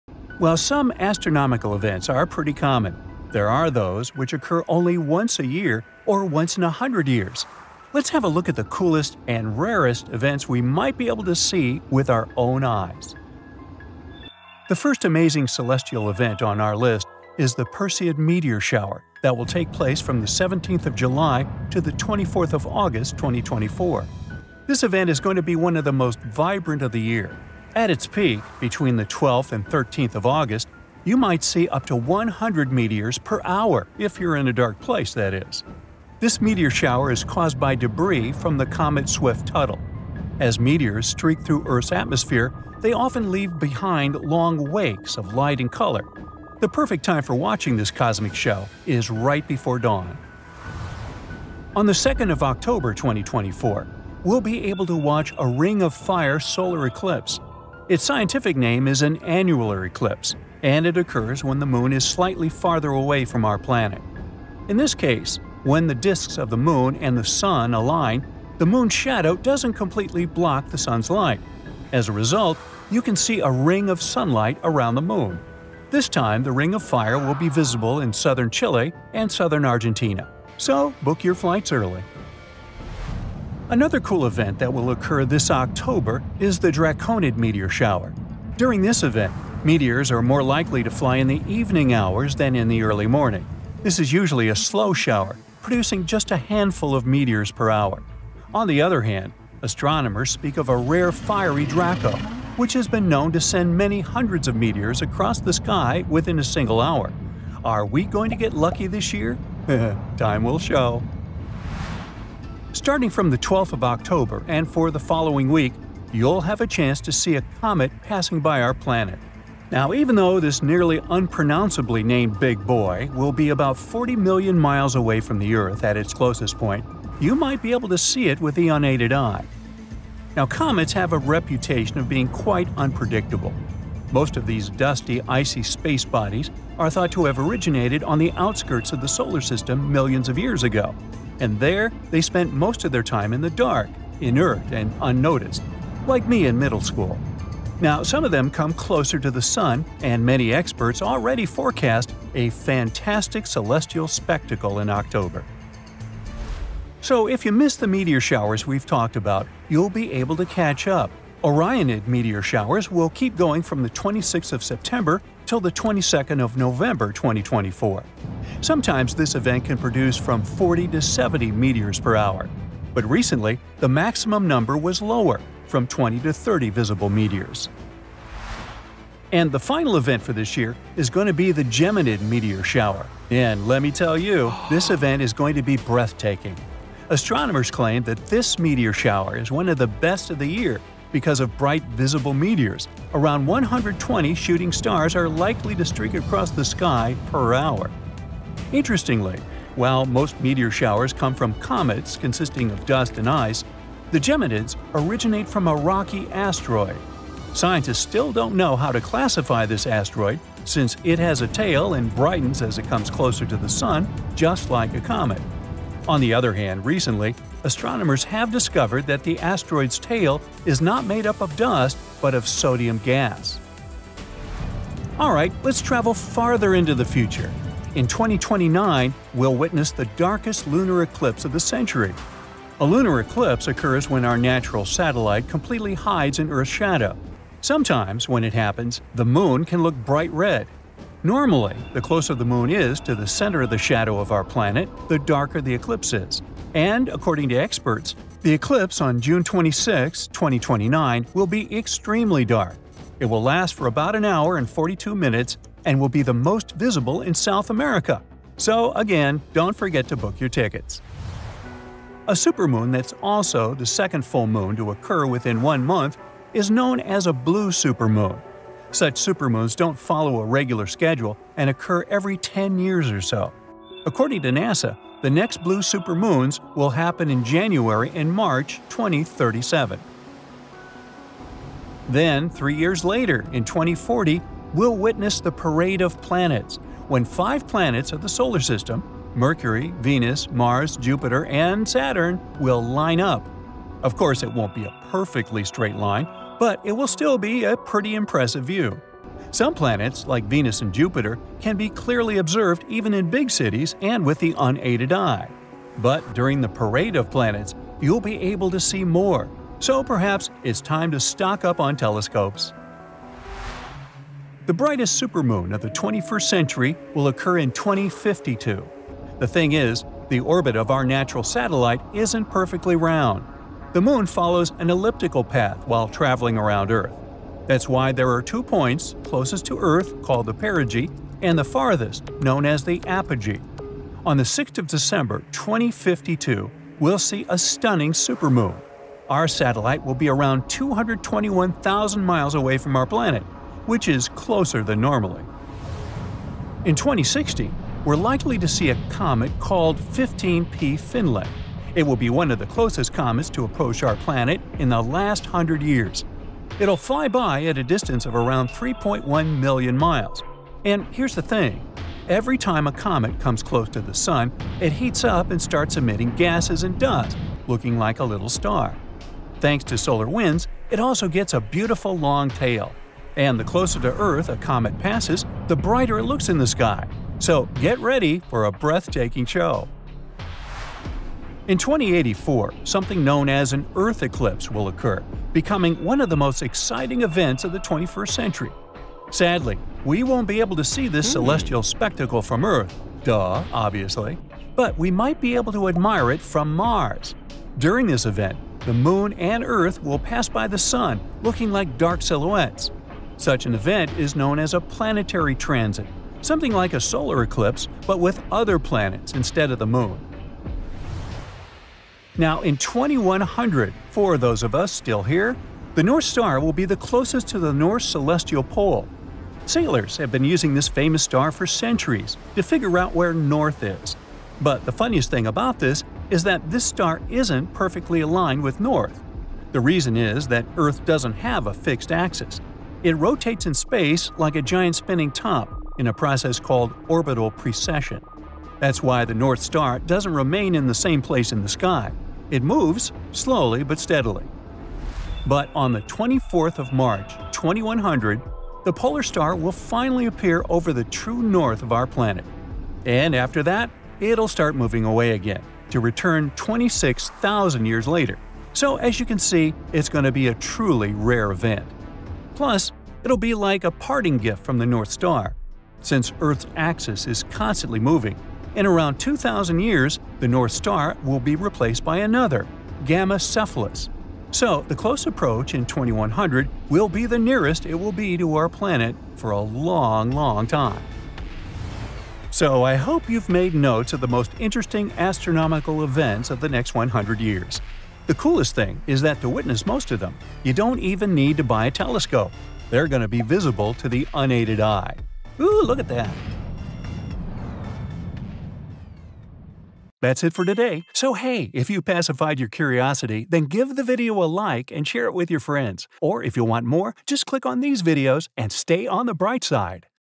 Meteor Showers and Other Upcoming Sky Events to Chase
Hey, stargazers! Get ready for some awesome sky events coming up. You won't want to miss the next meteor shower, and keep an eye out for the blue moon and supermoon that will light up the night sky. The Perseid meteor shower is also around the corner, and it's always a spectacular show. Plus, NASA has some exciting updates on a solar eclipse happening soon. These events remind us just how amazing our solar system and space really are!